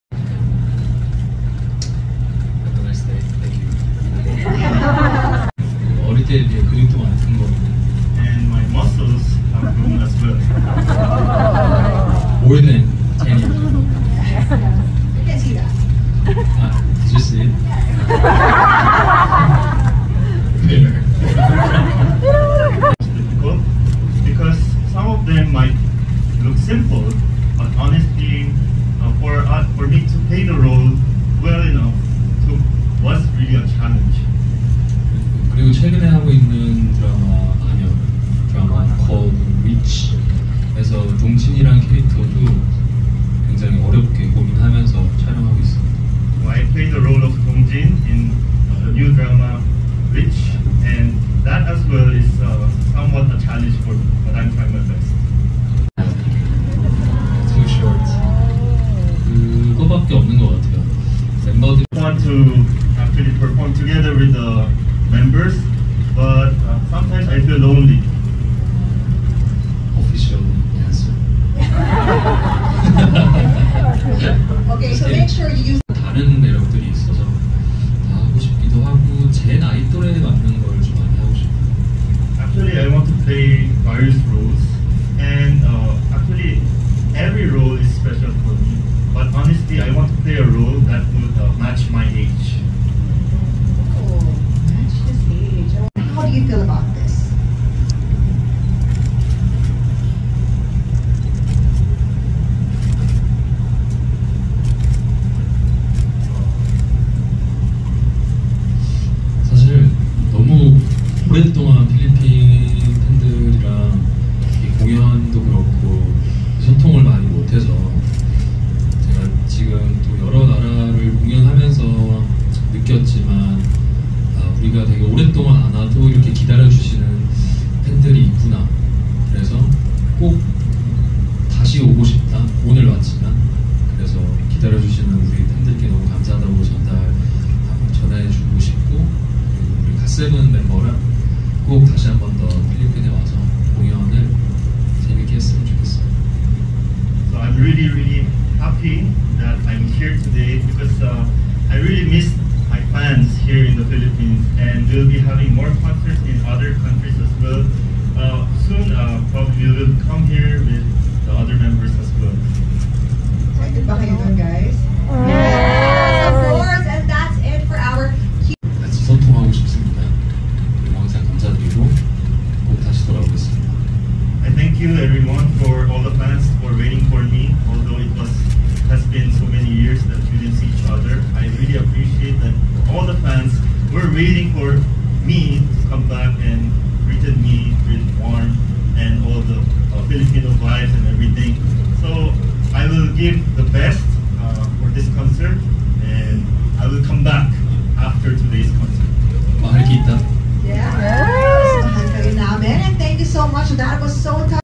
GOT7 Park Jin Young In Manila Press Conference Highlights 2023
ParkJinyoung Manila press conference highlights: talked about GOT7, his old and upcoming dramas, his message for his Filipino fans, etc. 💚